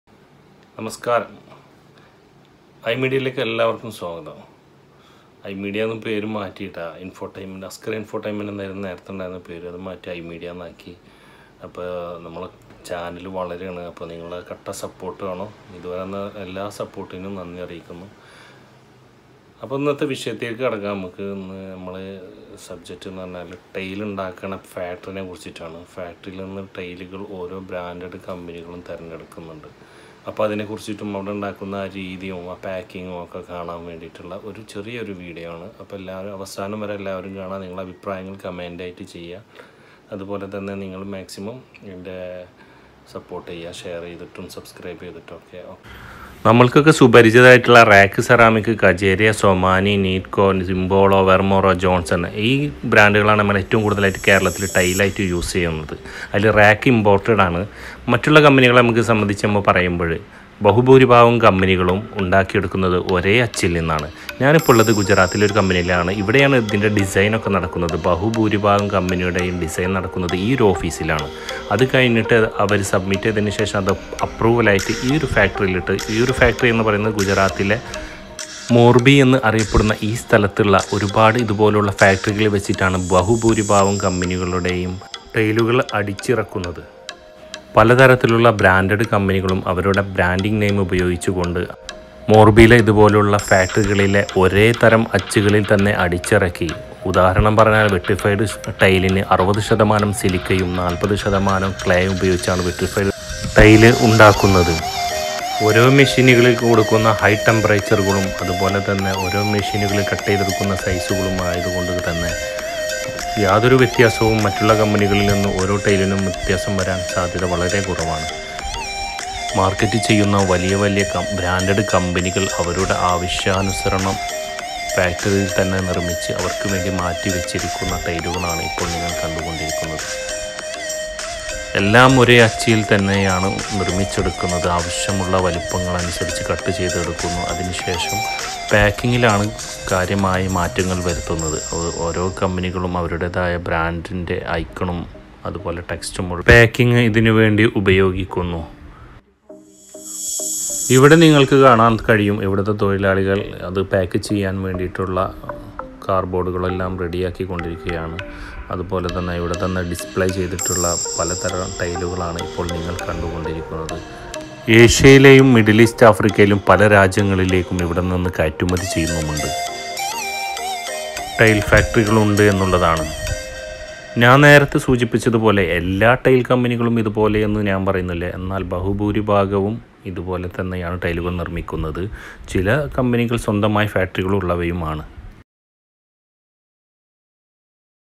മിക്ക ബ്രാൻഡ് ടൈൽസും വരുന്നത് ഇവിടെനിന്നും ഗുജറാത്ത് മോർബിയിലെ TILES ഉണ്ടാകുന്ന Factory കാണാം
@Askar infotainment
presents
ധാരാളം കമ്പനികൾ ടൈൽസ് മറ്റു പല ഫാക്ടറി കലിൽനിന്നും നിർമിച്ചു ബ്രാൻഡ് പതിപ്പിച്ചാണ് ഇറക്കുന്നത് അത്തരത്തിൽ ഉള്ള ഗുജറാത്തിലെ ഒരു ഫാക്ടറി കാണം